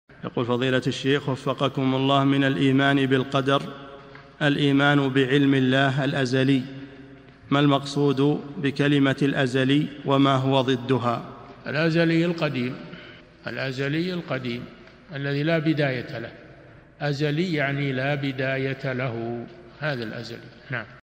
[458 -1024] ما المقصود بعلم الله الأزلي؟ - الشيخ صالح الفوزان
-[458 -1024] السؤال: من الإيمان بالقدر الإيمان بعلم الله الأزلي ، ما المقصود بكلمة الأزلي وما هو ضدها؟
الجواب: الأزلي القديم، الذي لا بداية له أزلي يعني لا بداية له هذا الأزلي.
للاشتراك في قناة الشيخ صالح الفوزان
سؤال وجواب من شرح الفتوى الحموية ( 1024 ) مقطع

للاستفسار والإقتراحات والدعم
رئيس الفريق
لمشروع كبار العلماء